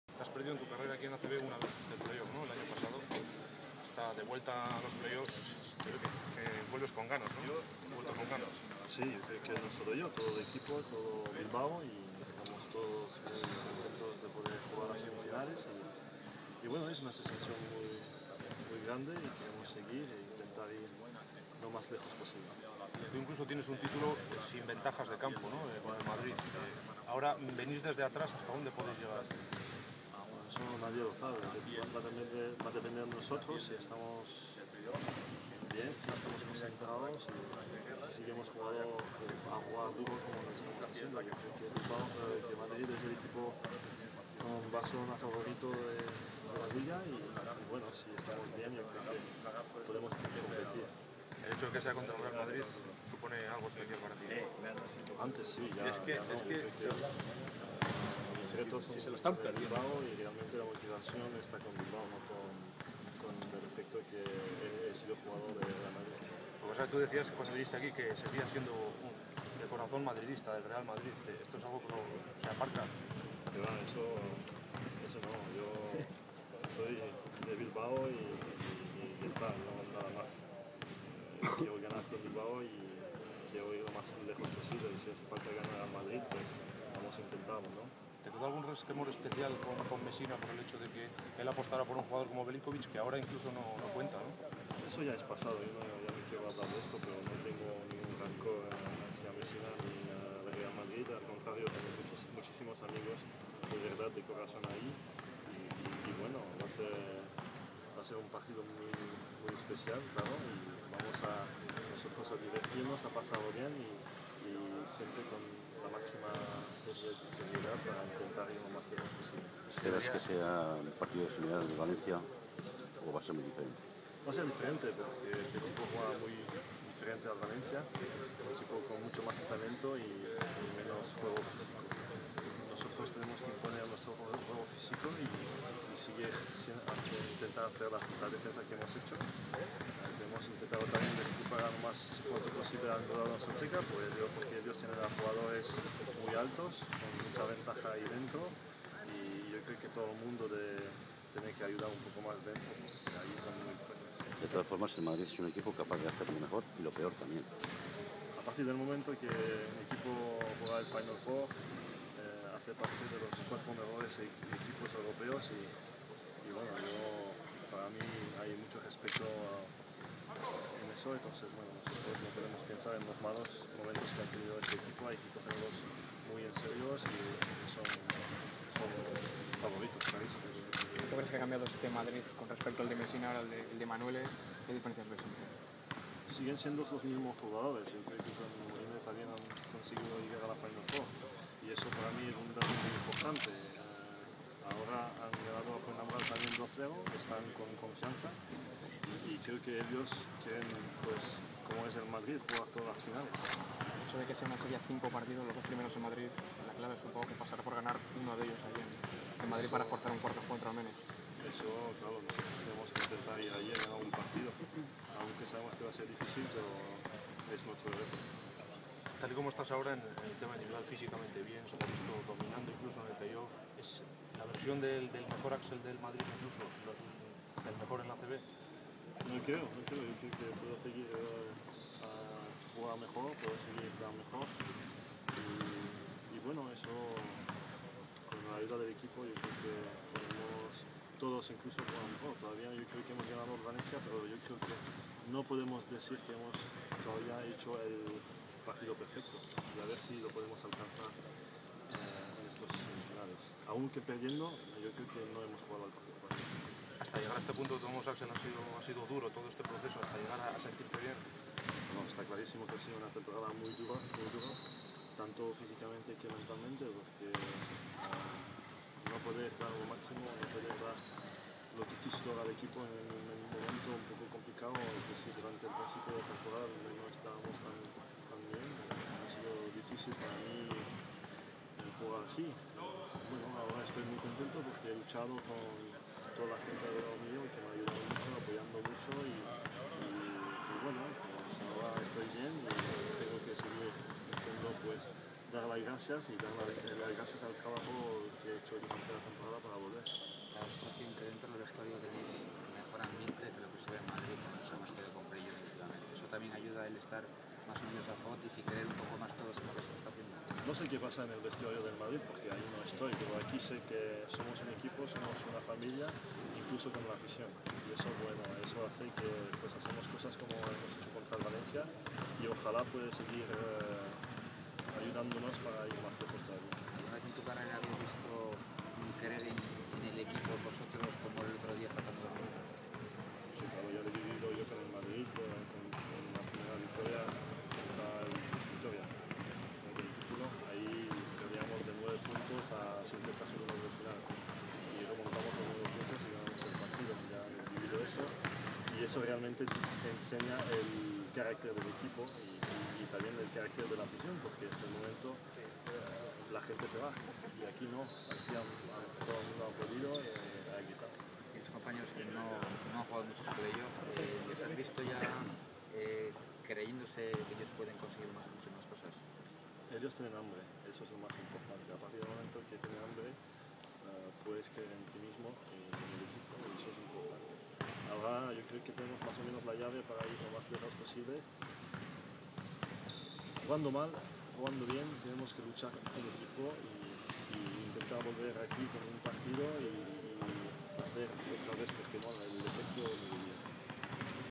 Axel Hervelle 05/24/2011
Declaraciones del jugador belga en la previa de las semifinales del play off frente al Real Madrid - Captured Live on Ustream with the Ustream Mobile App